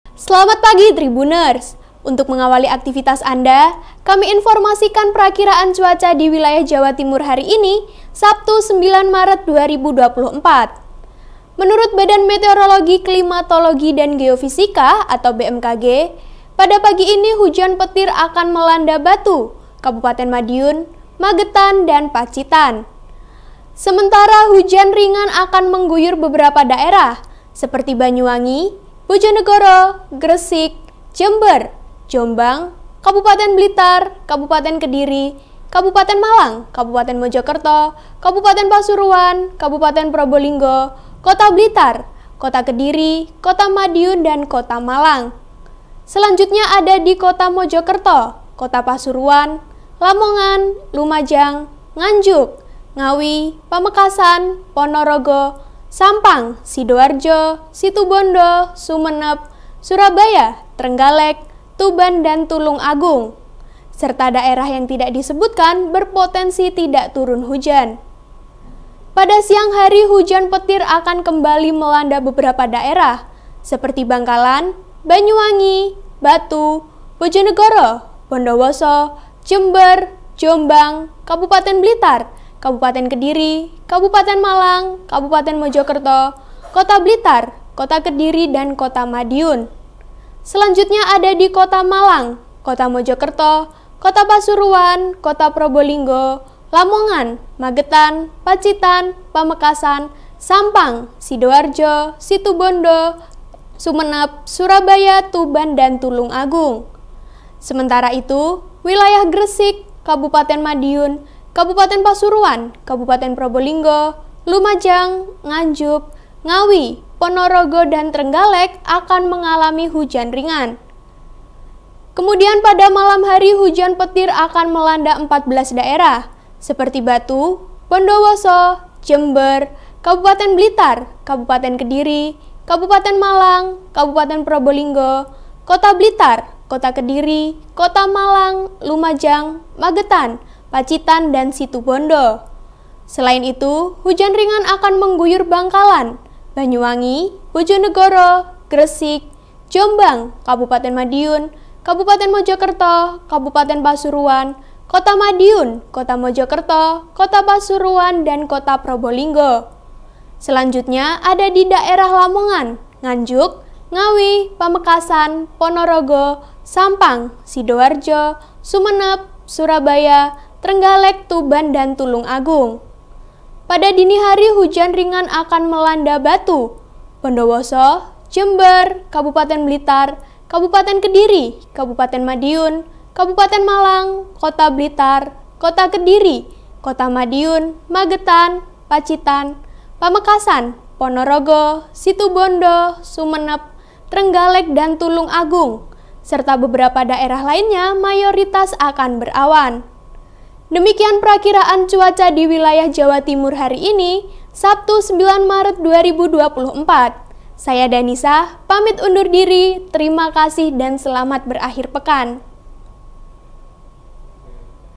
INFO CUACA SABTU 9 MARET 2024
VP: SUGIX

SURYA.CO.ID - Selamat Pagi Tribunners   untuk mengawali aktivitas anda   kami informasikan prakiraan cuaca di wilayah Jawa Timur hari ini    Sabtu 9 Maret 2024

Menurut Badan Meteorologi Klimatologi dan Geofisika (BMKG)   pada pagi ini hujan petir akan melanda Batu   Kabupaten Madiun   Magetan   dan Pacitan

Sementara hujan ringan akan mengguyur beberapa daerah   seperti Banyuwangi   Bojonegoro   Gresik   Jember   Jombang   Kabupaten Blitar   Kabupaten Kediri   Kabupaten Malang   Kabupaten Mojokerto   Kabupaten Pasuruan   Kabupaten Probolinggo   Kota Blitar   Kota Kediri   Kota Madiun   dan Kota Malang

Selanjutnya ada di Kota Mojokerto   Kota Pasuruan   Lamongan   Lumajang   Nganjuk   Ngawi   Pamekasan   Ponorogo   Sampang   Sidoarjo   Situbondo   Sumenep   Surabaya   Trenggalek   Tuban   dan Tulungagung   serta daerah yang tidak disebutkan   berpotensi tidak turun hujan

Pada siang hari hujan petir akan kembali melanda beberapa daerah   seperti Bangkalan   Banyuwangi   Batu   Bojonegoro   Bondowoso   Jember   Jombang   Kabupaten Blitar   Kabupaten Kediri   Kabupaten Malang   Kabupaten Mojokerto   Kota Blitar   Kota Kediri   dan Kota Madiun

Selanjutnya ada di Kota Malang   Kota Mojokerto   Kota Pasuruan   Kota Probolinggo   Lamongan   Magetan   Pacitan   Pamekasan   Sampang   Sidoarjo   Situbondo   Sumenep   Surabaya   Tuban   dan Tulungagung

Sementara itu di wilayah Gresik   Kabupaten Madiun   Kabupaten Pasuruan   Kabupaten Probolinggo   Lumajang   Nganjuk   Ngawi   Ponorogo   dan Trenggalek   akan mengalami hujan ringan

Kemudian pada malam hari hujan petir akan melanda 14 daerah   seperti Batu   Bondowoso   Jember   Kabupaten Blitar   Kabupaten Kediri   Kabupaten Malang   Kabupaten Probolinggo   Kota Blitar   Kota Kediri   Kota Malang   Lumajang   Magetan   Pacitan   dan Situbondo

Demikian prakiraan cuaca di wilayah Jawa Timur hari ini   Sabtu 9 Maret 2024   Saya Danisa   pamit undur diri   terima kasih dan selamat berakhir pekan